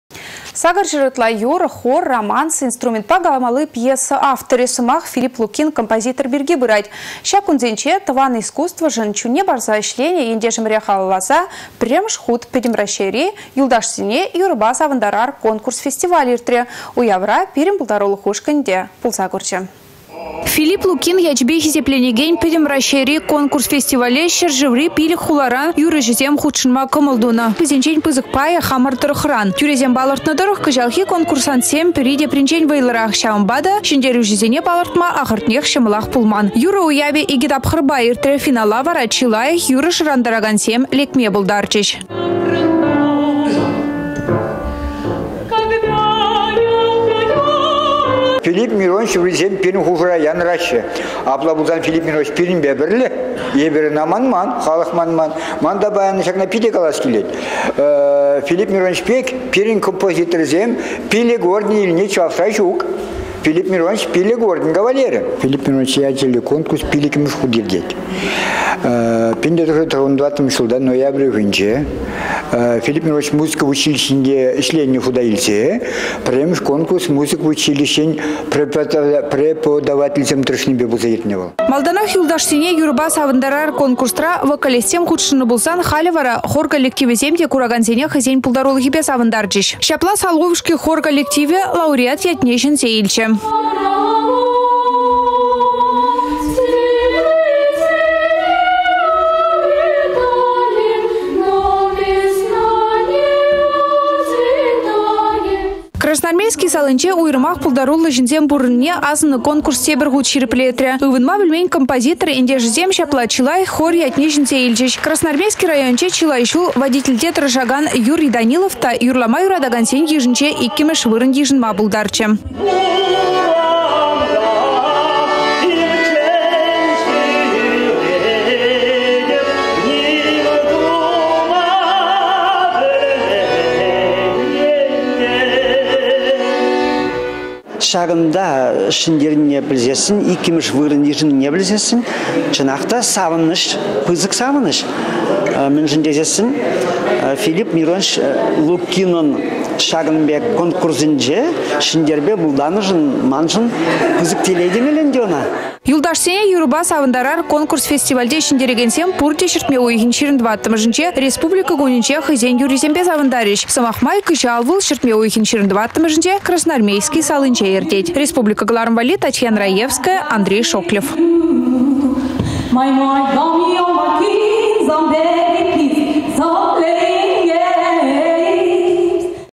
«Юлташсене юрăпа савăнтарар»
800 ытла юрă, хор, романс, инструментпа каламалли пьеса авторĕ. Сăмах Филипп Лукин композитор пирки пырать. Çак кунсенче тăван искусствăшăн чунне парса ĕçленĕ ентешĕмĕре халалласа пĕрремĕш хут пĕтĕм Раççейри  «Юлташсене юрăпа савăнтарар» конкурс-фестиваль иртрĕ. Уявра пирĕн пултарулăх ушкăнĕ те пулса курчĕ.